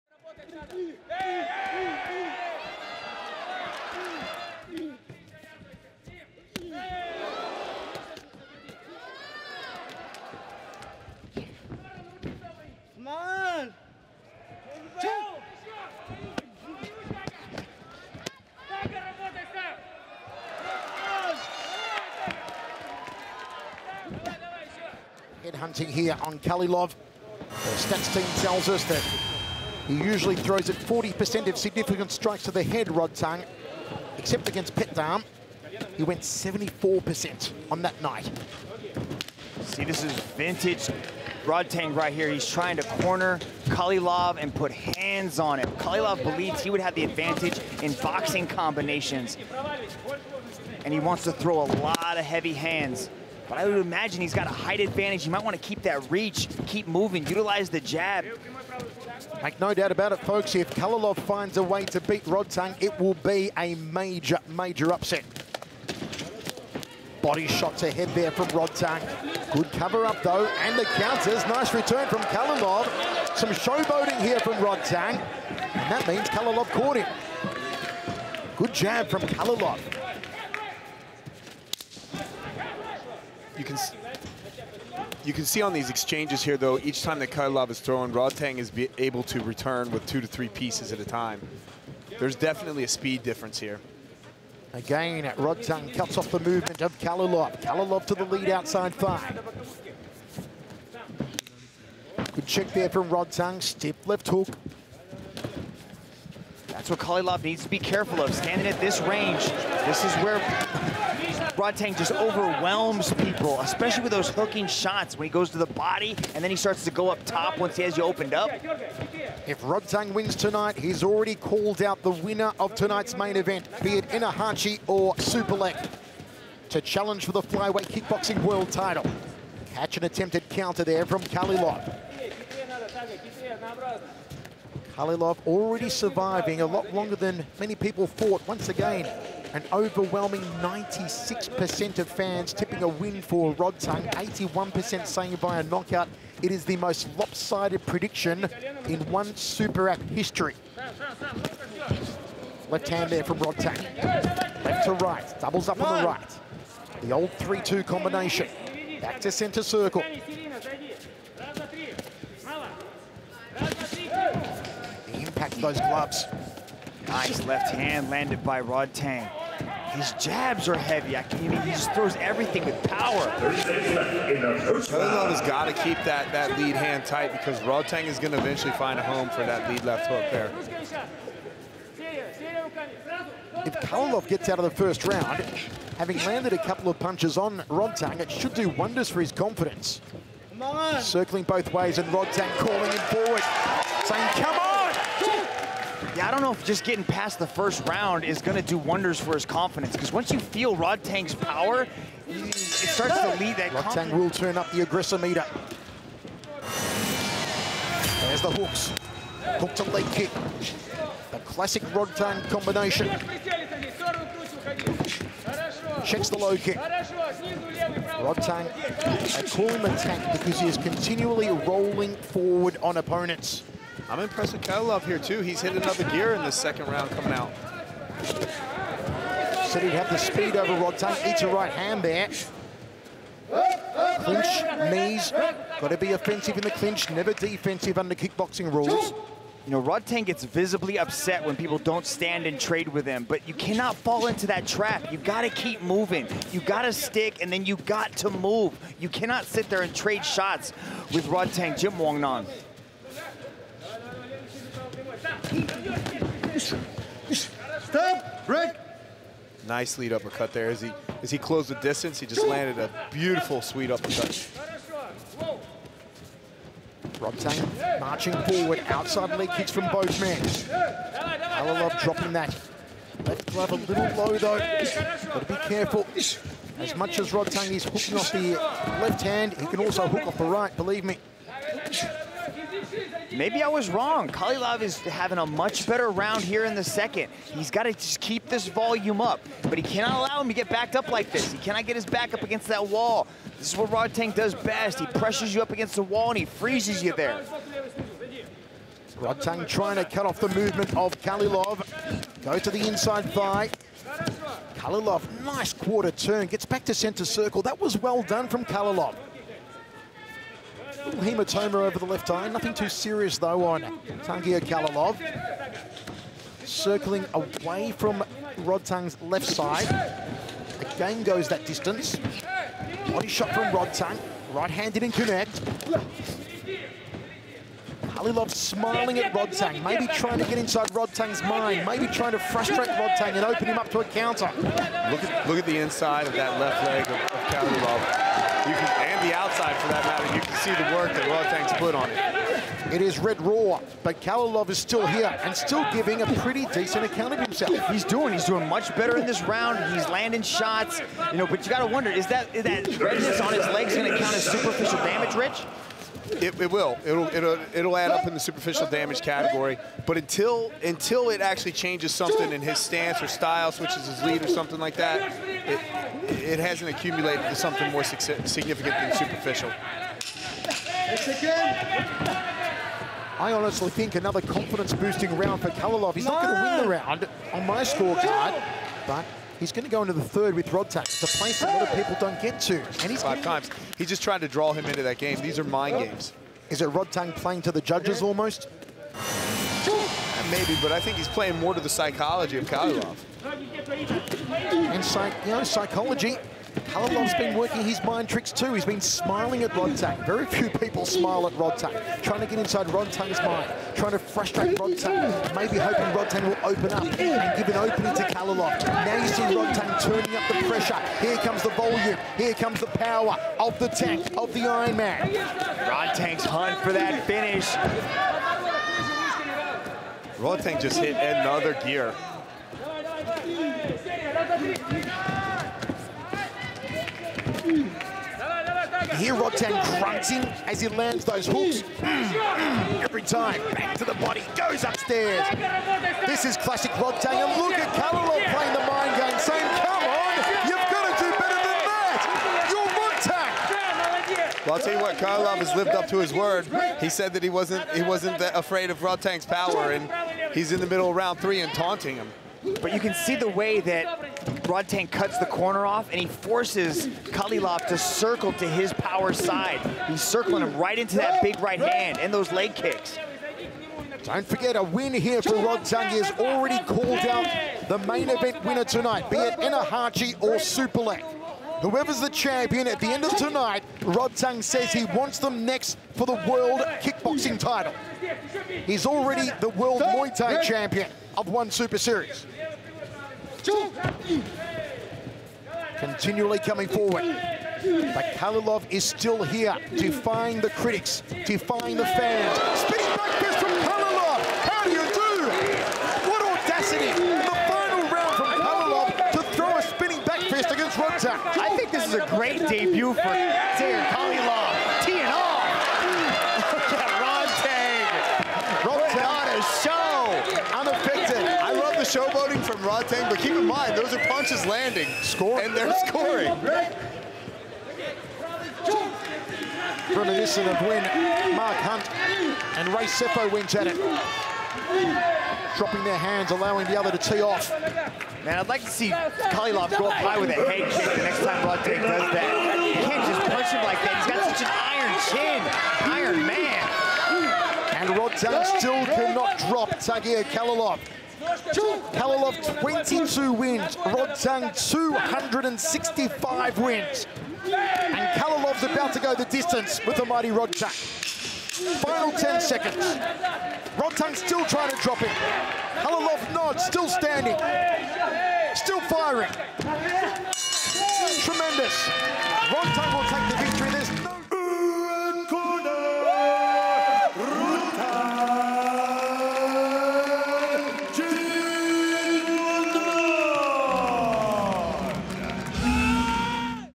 Kickboxing RAMPAGE 👊🔥 Rodtang vs. Tagir Khalilov Full Fight
Get hyped for flyweight Muay Thai king Rodtang's World Title defense against Mexican debutant Edgar Tabares at ONE Fight Night 10 on Prime Video on May 5 by reliving his kickboxing firefight with Russian striking sensation Tagir Khalilov in 2021!

ONEFightNight10 | Friday at 8PM ET/5PM PT